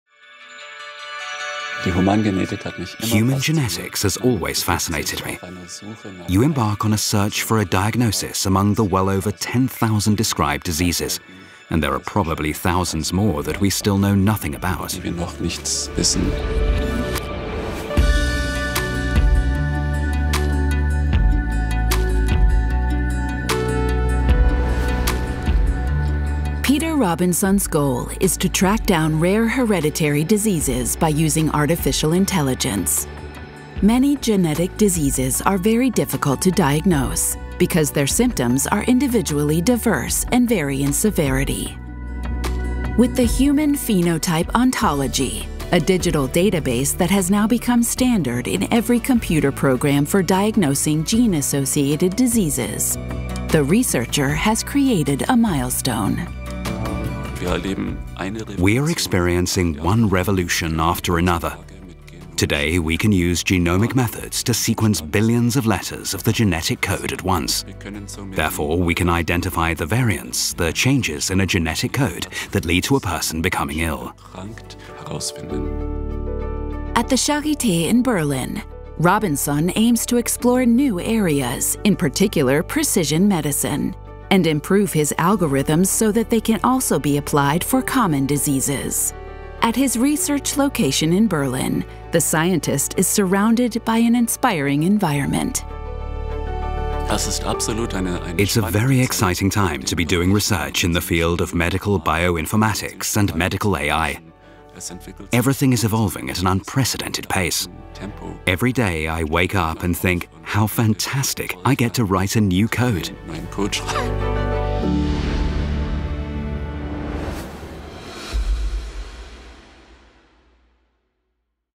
AI-based precision medicine | Humboldt professor for AI Peter N. Robinson
Diseases like flu and chickenpox are relatively easy to diagnose. It is known that symptoms like fever, aching limbs, a cough or a characteristic skin rash can be traced back to a specific pathogen. It is much more difficult when it comes to the early diagnosis of genetic diseases – because gene mutation can lead to symptoms of varying severity. And the clinical manifestations often differ, as well. Traditionally, some were not linked to genetic deviations at all, which meant hereditary diseases remained unidentified.

In 2008, the medical researcher and genome expert, Peter N. Robinson, developed the Human Phenotype Ontology of genetically associated diseases. The database allows users to match the clinical disease manifestations with the relevant gene mutations and syndromes. This computer assisted reference source is continuously updated and now contains 13,000 terms and 156,000 annotations on genetic diseases. Various applications mean the data can be searched and weighted according to the information provided by the user - thus facilitating diagnosis.

With the database Robinson laid the foundations for tracing rare inherited diseases with the help of artificial intelligence. He is a pioneer of computer-assisted phenotype analysis and has been one of the top researchers in the field for many years. He himself has identified several genes associated with Mendelian genetic diseases and written algorithms that facilitate the investigation of genome and exome sequences as well as finding correlations with clinical manifestations and gene mutations.

With the arrival of Peter N. Robinson, the Berlin Institute of Health at Charité (BIH) will recruit an excellent protagonist of translational research in bioinformatics who is supposed to close the gap between data science and applied medicine. In the long term, this should facilitate tailored precision therapy because the computer-assisted data analysis methods mean patient data on genetic diseases can be clustered, data compared, clinical procedures predicted and personalised treatment strategies developed.